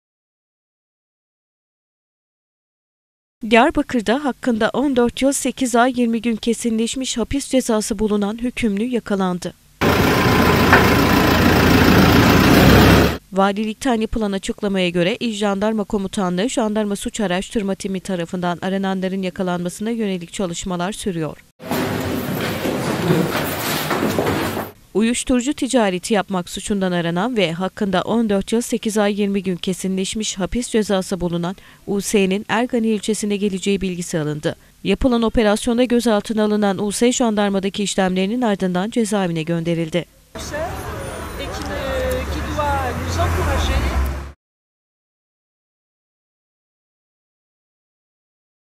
KESİNLEŞMİŞ HAPİS CEZASI BULUNAN HÜKÜMLÜ YAKALANDI
Diyarbakır'da hakkında 14 yıl 8 ay 20 gün kesinleşmiş hapis cezası bulunan hükümlü yakalandı.

Valilikten yapılan açıklamaya göre, İl Jandarma Komutanlığı Jandarma Suç Araştırma Timi (JASAT) tarafından arananların yakalanmasına yönelik çalışmalar sürüyor.

"Uyuşturucu ticareti yapmak" suçundan aranan ve hakkında 14 yıl 8 ay 20 gün kesinleşmiş hapis cezası bulunan U.S'nin Ergani ilçesine geleceği bilgisi alındı.

Yapılan operasyonda gözaltına alınan U.S. jandarmadaki işlemlerinin ardından cezaevine gönderildi.